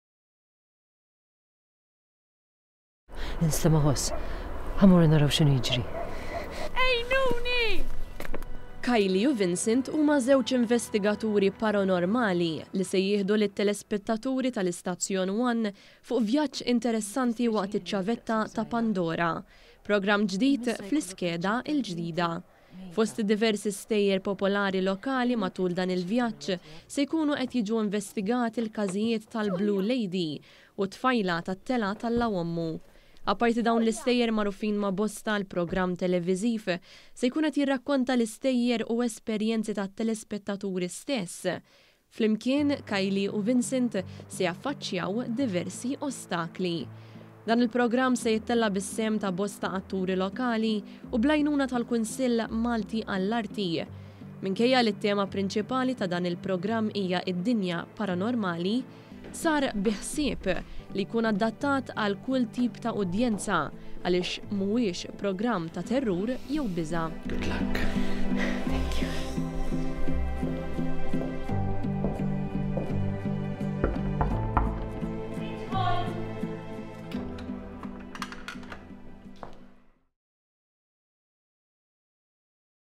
ID-DINJA PARANORMALI INVESTIGATA FI PROGRAMM ĠDID FUQ ONE
ONE.COM.MT • Mil-lejla fuq ONE se jibda jixxandar dramm ġdid bl-isem ta’ “iċ-Ċavetta ta’ Pandora”.
Matul l-iskeda tax-Xitwa, iċ-Ċavetta ta’ Pandora se jeħodkom fuq vjaġġ paranormali li se jinvestiga stejjer popolari Maltin relatati mal-ispirti.

KITBA: ONE NEWS
FILMATI: ONE

Kylie u Vincent huma żewġ investigaturi paranormali li se jieħdu lit-telespettaturi tal-istazzjon ONE fuq vjaġġ interessanti waqt iċ-Ċavetta ta’ Pandora; programm ġdid fl-iskeda l-ġdida.

Fost id-diversi stejjer popolari lokali, matul dan il-vjaġġ se jiġu investigati l-każijiet tal-Blue Lady u t-tfajla tat-Telgħa t’Alla u Ommu.

Apparti dawn l-istejjer magħrufin ma’ bosta, il-programm televiżiv se jkun qed jirrakkonta l-istejjer u esperjenza tat-telespettaturi stess.

Flimkien, Kylie u Vincent se jaffaċċjaw diversi ostakli.

Dan il-programm se jittella’ bis-sehem ta’ bosta atturi lokali u bl-għajnuna tal-Kunsill Malti għall-Arti.
Minkejja li t-tema prinċipali ta’ dan il-programm hija d-dinja paranormali, sar bi ħsieb li jkun adattat għal kull tip ta’ udjenza għaliex mhuwiex programm ta’ terrur jew biża’.